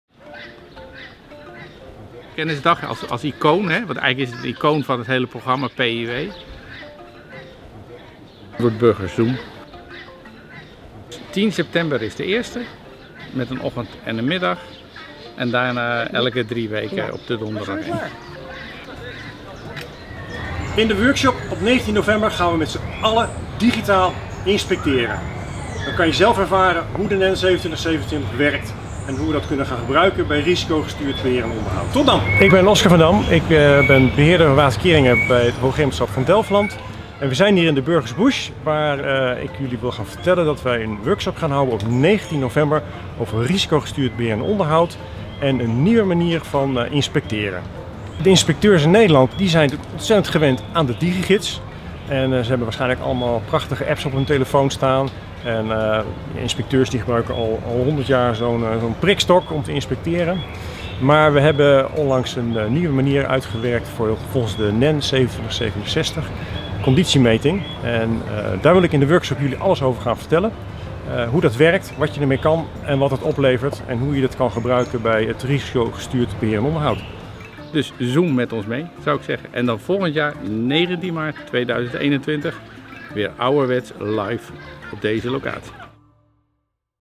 Waterveiligheid | Teaser Kennisdag Inspectie Waterkeringen/risicogestuurd beheer en onderhoud | 2020
In 2018 verscheen de handreiking Risicogestuurd beheer en onderhoud van keringen, met daarin een voorstel om de diagnose en prognose geprioriteerd te kunnen uitvoeren. En daarvoor is een andere manier van het registreren van inspecties nodig: de NEN2767 conditiemeting. Het afgelopen half jaar heeft een expertgroep intensief het beheerobject Dijk voor de NEN2767 uitgewerkt. In de workshop op 19 november 2020 verlet objectbeheerder waterkeringen Oscar van Dam meer over de resultaten, ga je er zelf mee aan de slag en ervaar je hoe je dat kunt inzetten voor risicogestuurd beheer en onderhoud.